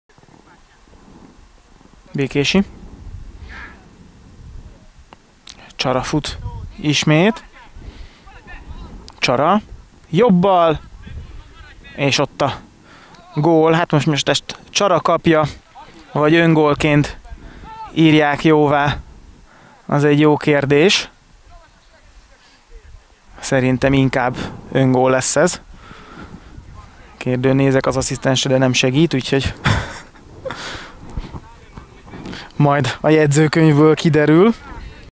2018.12.02. Domaszék - Újszentiván 3-6, Jaksa Tibor öngólja (3-5)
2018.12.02. Domaszék SK - Újszentiván SE mérkőzés, Csongrád megyei 4. osztály, Csara Ferenc lövése Jaksa Tiborról vágódik a kapuba
© Móra-net TV, 2018.
Kommentátor: dr. Németh László, operatőr: Szécsi Gábor